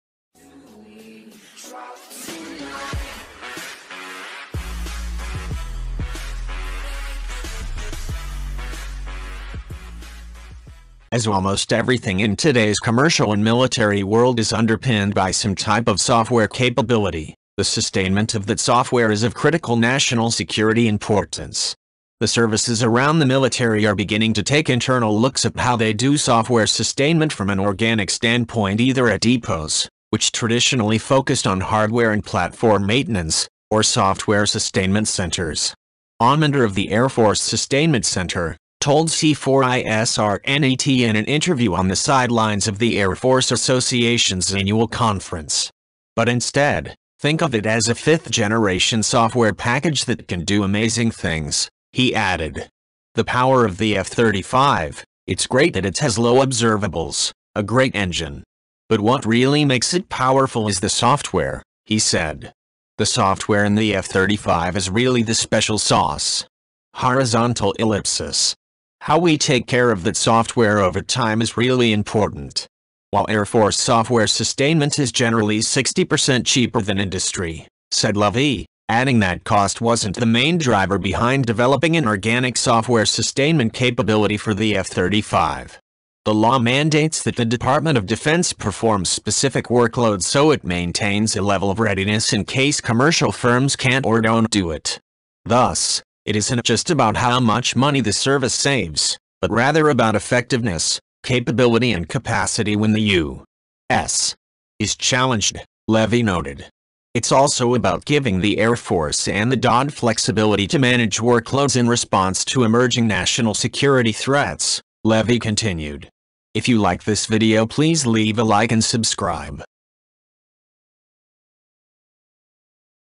F 35’s ‘SPECIAL SAUCE’ Air Force developing software sustainment for jet
As almost everything in today’s commercial and military world is underpinned by some type of software capability, the sustainment of that software is of critical national security importance.

The services around the military are beginning to take internal looks at how they do software sustainment from an organic standpoint either at depots — which traditionally focused on hardware and platform maintenance — or software sustainment centers.ommander of the Air Force Sustainment Center, told C4ISRNET in an interview on the sidelines of the Air Force Association’s annual conference. But instead, think of it as a fifth-generation software package that can do amazing things, he added.

The power of the F-35, it’s great that it has low observables, a great engine. But what really makes it powerful is the software,  he said. “The software in the F-35 is really the special sauce. … How we take care of that software over time is really important.

While Air Force software sustainment is generally 60 percent cheaper than industry, said Levy, adding that cost wasn’t the main driver behind developing an organic software sustainment capability for the F-35.The law mandates that the Department of Defense performs specific workloads so it maintains a level of readiness in case commercial firms can’t or don’t do it. Thus, it isn’t just about how much money the service saves, but rather about effectiveness, capability and capacity when the U.S. is challenged, Levy noted. It’s also about giving the Air Force and the DoD flexibility to manage workloads in response to emerging national security threats, Levy continued.